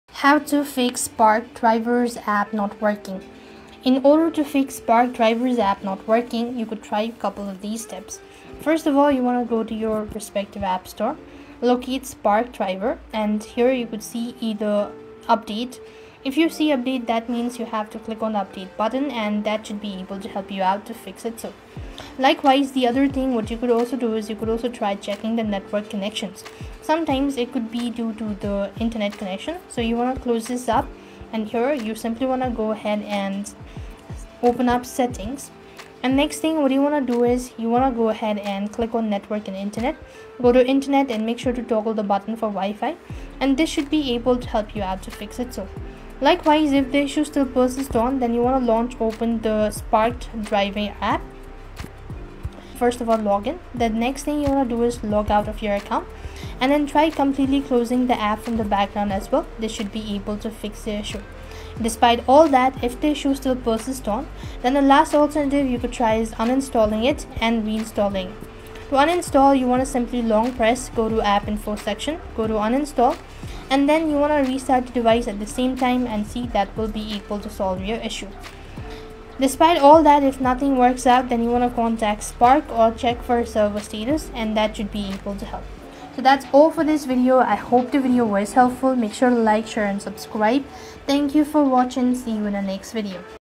✅ How to Fix Spark Driver App Not Working
If your Spark Driver app is not working properly, there are several common troubleshooting steps you can take to resolve the issue. This guide walks you through simple methods that often fix bugs, connectivity issues, or performance problems in the app. Spark Driver, which supports Walmart's Spark delivery network, relies on timely updates and a stable internet connection to function correctly. Here's how to troubleshoot if it's not responding or crashing.

Start by checking for updates on your device’s app store. An outdated version of the Spark Driver app can lead to bugs or connectivity problems. Open the App Store (iOS) or Google Play Store (Android), search for "Spark Driver," and tap 'Update' if it’s available. Keeping your app up-to-date ensures you're running the most stable and secure version.

Next, verify your internet connection. Poor or unstable connections are a common cause of app malfunction. Head into your phone’s 'Settings,' navigate to 'Network & Internet,' and toggle your Wi-Fi off and on to reset the connection. Try switching to mobile data if Wi-Fi remains unresponsive.

If updating the app and checking connectivity don’t work, try restarting the Spark Driver app. Log in, then log out, and close the app from the background completely. Reopening it fresh often clears minor glitches.

Still not working? You can uninstall and reinstall the app. Long-press the app icon, go to 'App Info,' then select 'Uninstall.' Once done, restart your phone and reinstall Spark Driver from the app store.

As a final step, check Spark’s server status or contact Spark support. App-wide outages can prevent it from functioning, in which case only the platform’s support team can assist.

Timestamps:
0:00 Introduction
0:03 Check for app updates
0:24 Test network connection
0:50 Log out and restart app
1:06 Uninstall and reinstall
1:21 Contact Spark or check server
1:28 Conclusion